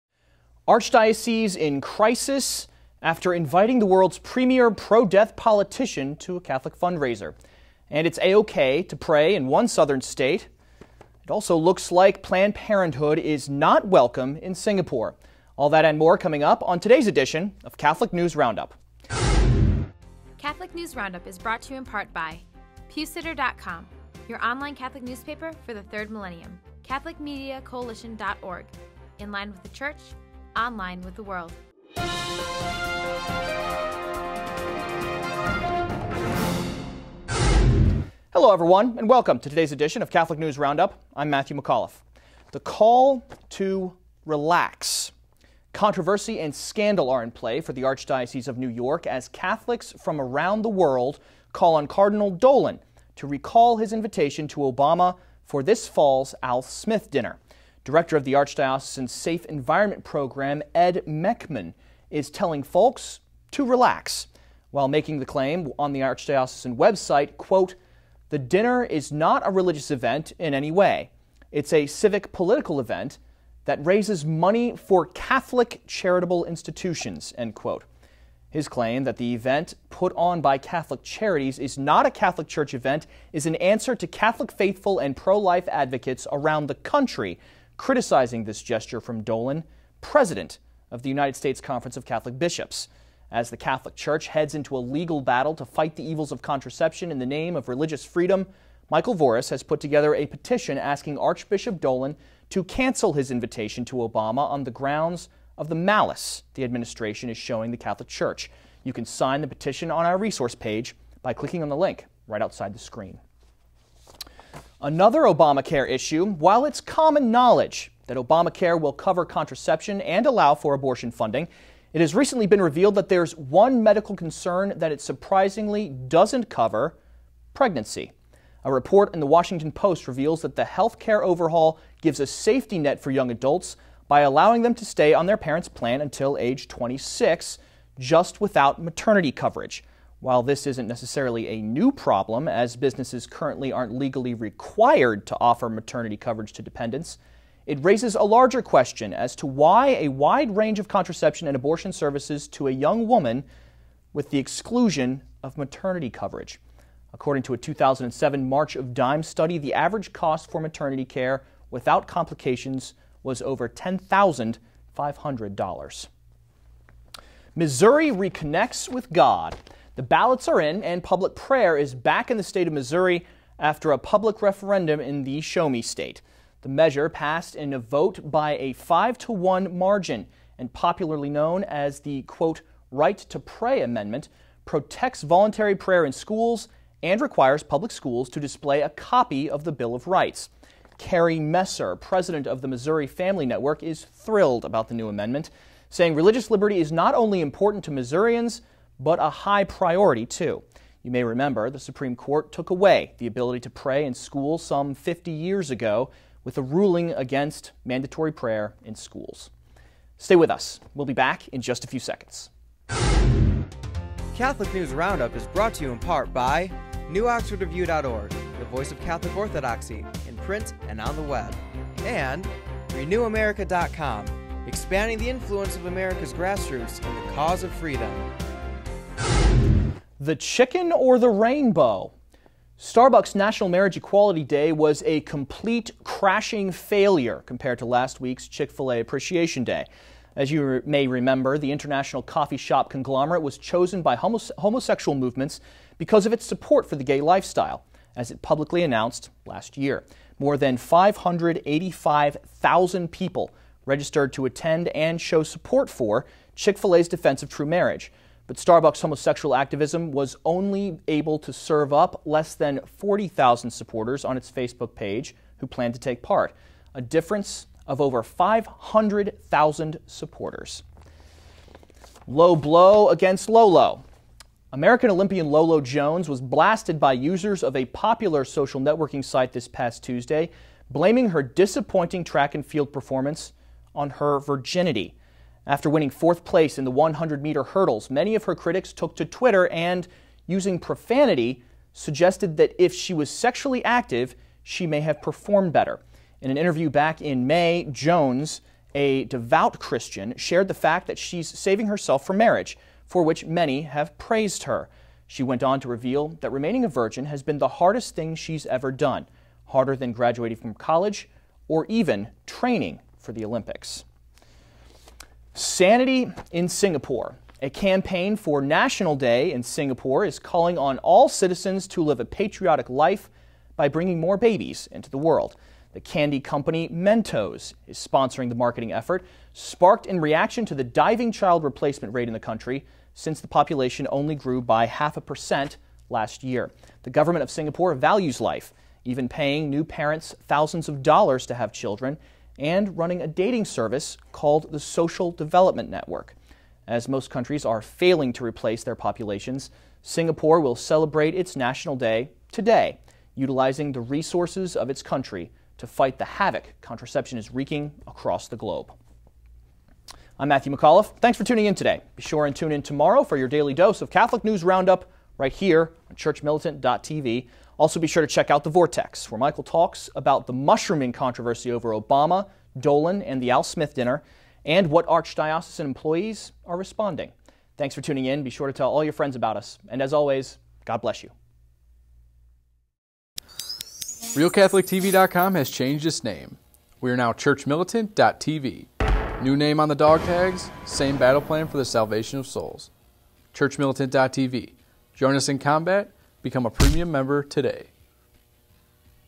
Catholic News Roundup 08-09
Today's stories -
- The Call To Relax
- Another Obamacare Issue
- Missouri Reconnects With God
- The Chicken Or The Rainbow
- Low Blow Against LoLo
- Sanity In Singapore